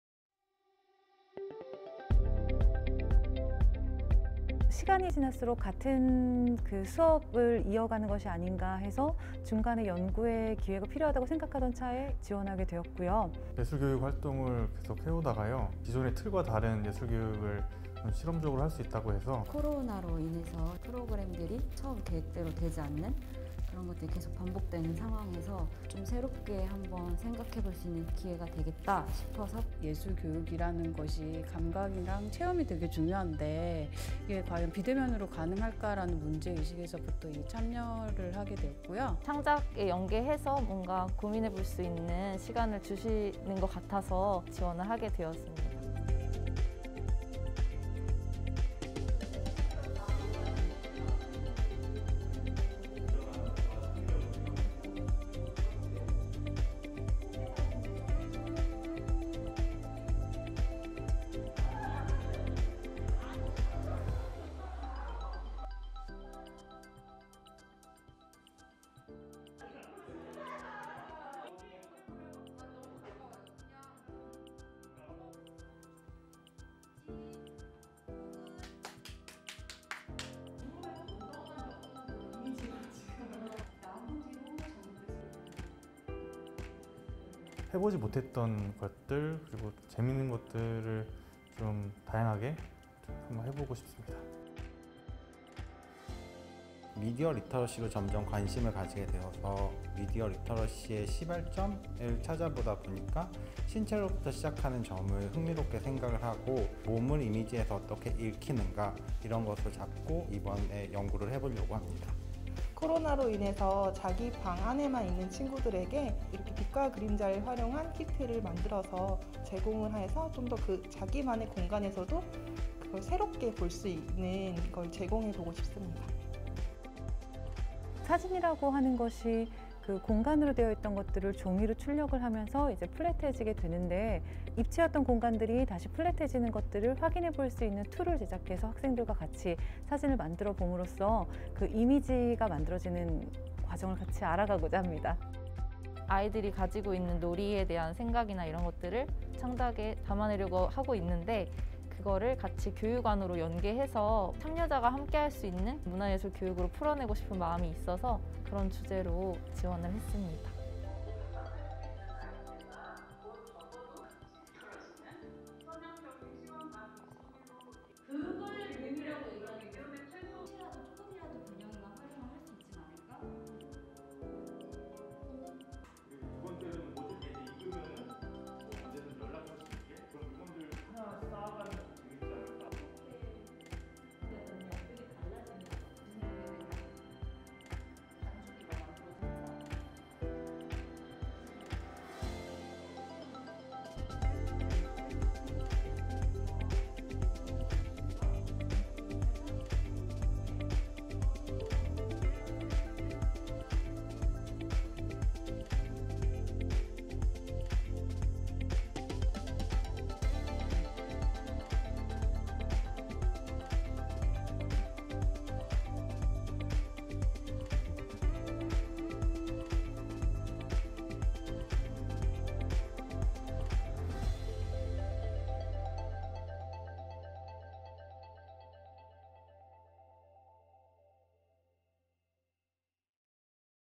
[2020 기획프로그램] 예술교육 창작연구 프로그램
한예종 문화예술교육센터에서 2020년도 기획프로그램으로 진행한 [예술교육 창작 연구 프로그램] 입니다.

참여한 5팀의 연구과정이 담긴 내용으로  컨셉화된 예술교육이 아닌 일상과 주변을 돌아보고, 예술가 개인의 창작 활동에 기반한 예술성, 예술적 경험을 반영한 예술교육 연구 프로그램입니다.